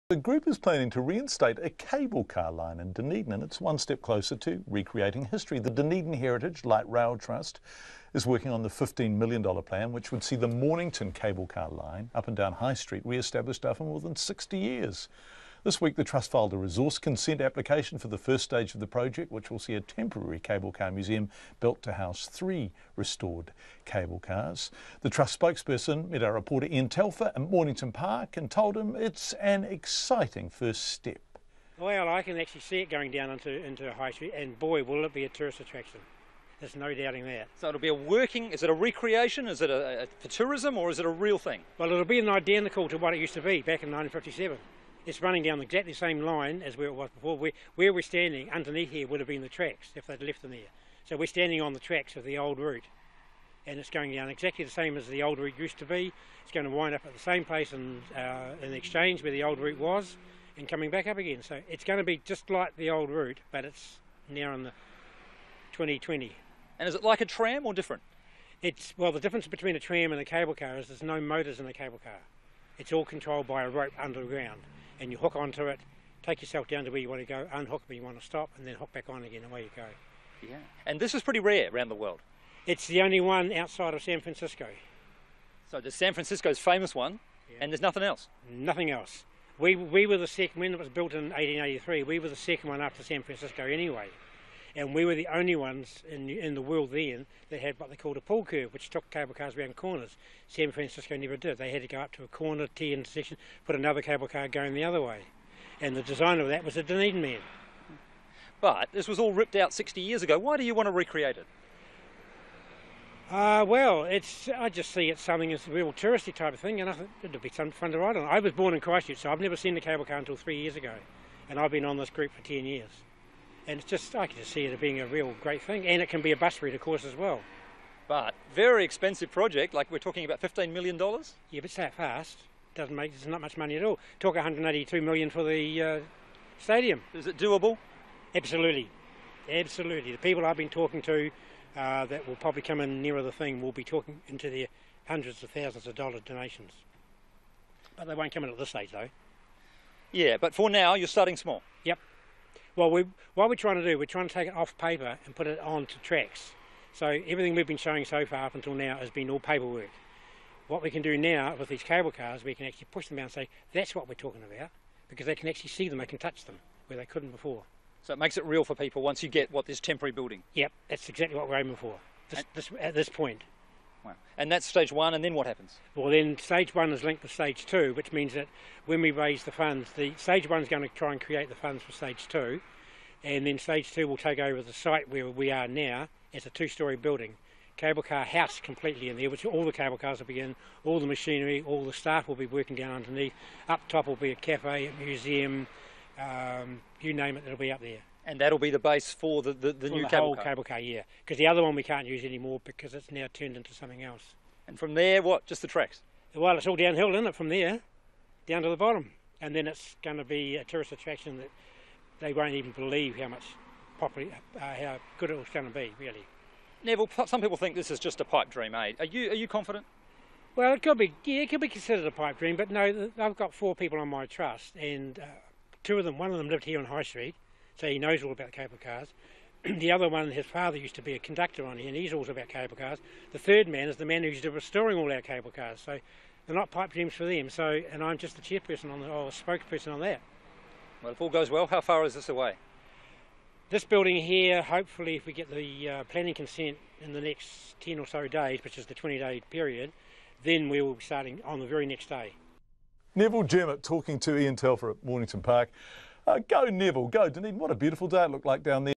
Dunedin cable car one step closer to reality
A group planning to reinstate a cable car line in Dunedin after more than 60 years is one step closer to recreating history.